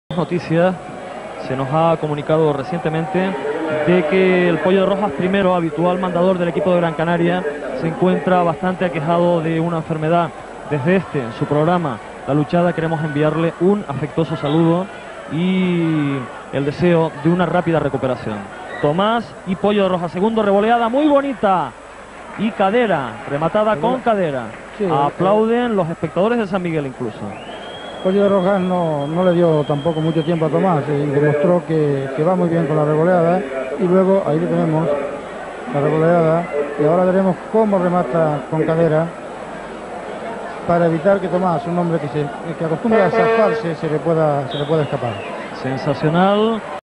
Francisco Rivero Pollo de Rojas II y Tomás González 2ª Ag. 15/12/1989
Francisco Rivero "Pollo de Rojas II" y Tomás González
2ª Agarrada en Luchada del Club de Luchas Chimbesque y el Club de Luchas Maspalomas de la "Liga Regional" en el Terrero Tomás González de San Miguel de Abona Tenerife el 15/12/1989.